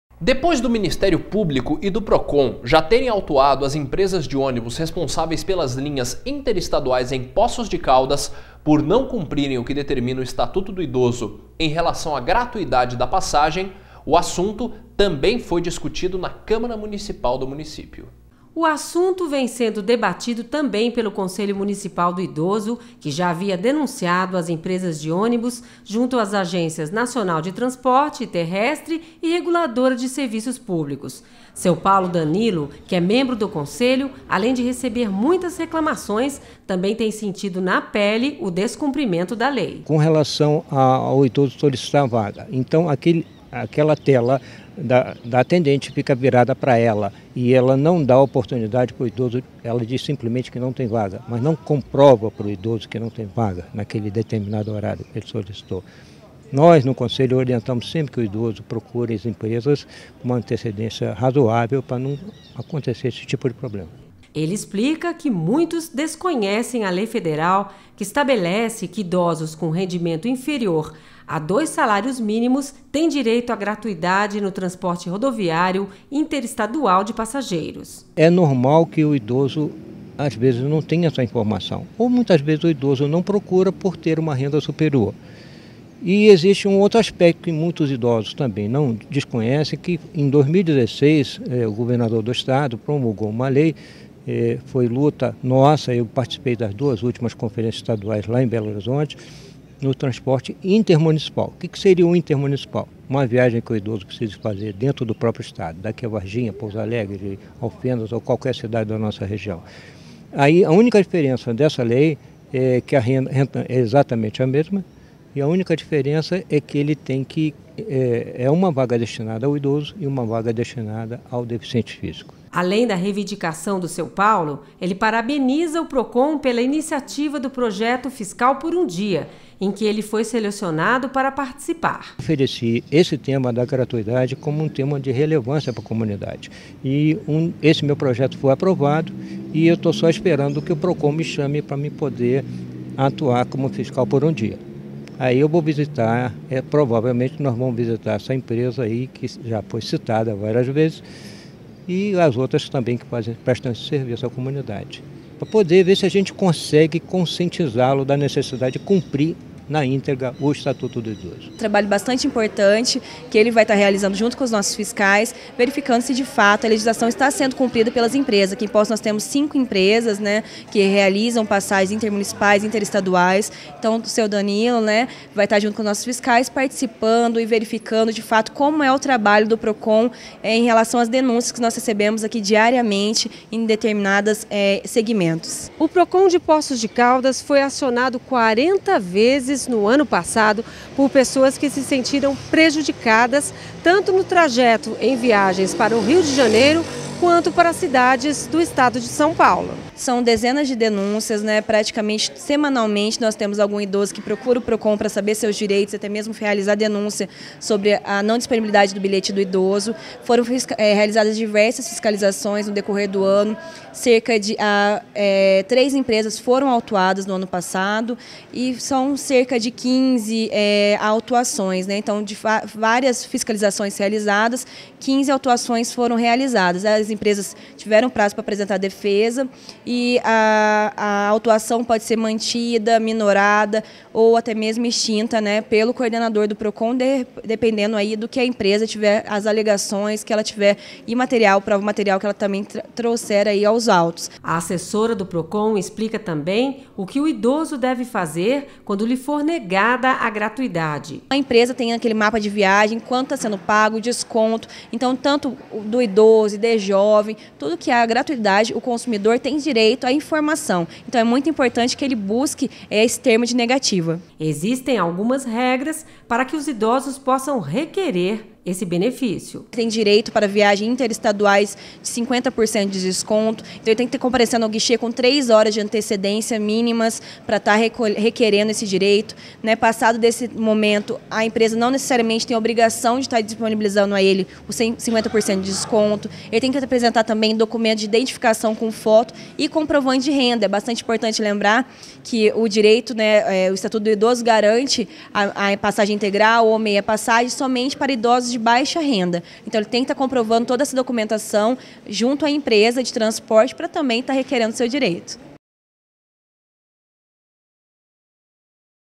Câmara de Poços de Caldas discute sanções a empresas que não cumprem lei da gratuidade para idosos
Depois do ministério público e Procon já terem autuado as empresas de ônibus responsáveis pelas linhas interestaduais em Poços de Caldas, por não cumprirem o que determina o estatuto do idoso em relação à gratuidade da passagem, o assunto também é discutido na câmara municipal da cidade.